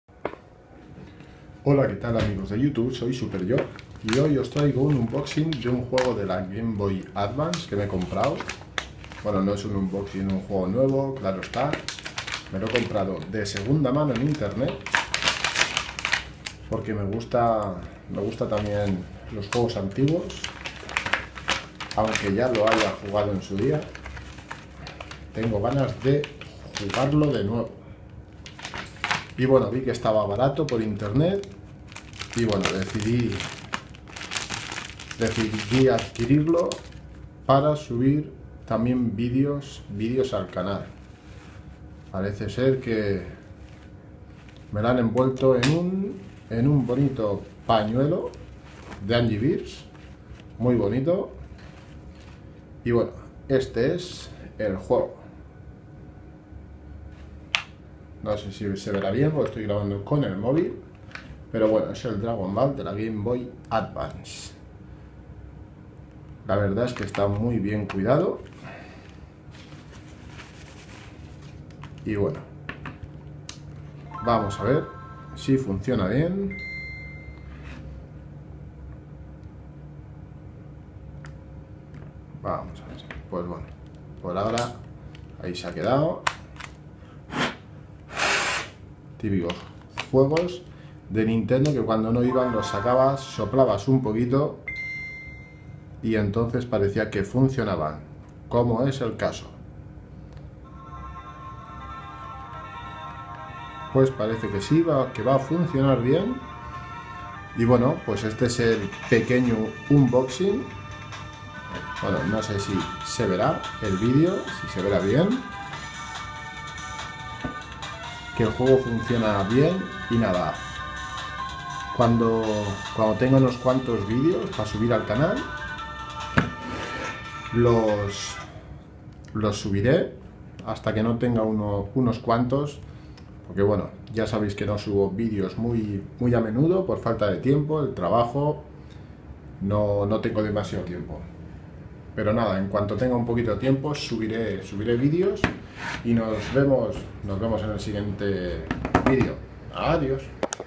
UNBOXING Dragon Ball El Legado De Goku
Hola amigos de YouTube,os traigo este unboxing de un juego de segunda mano que compre para rejugarlo ya que soy fan de Dragon Ball y asi poder hacer videos para el canal.Espero que os guste y nos vemos en el siguiente video aaaaaaadiossssss =P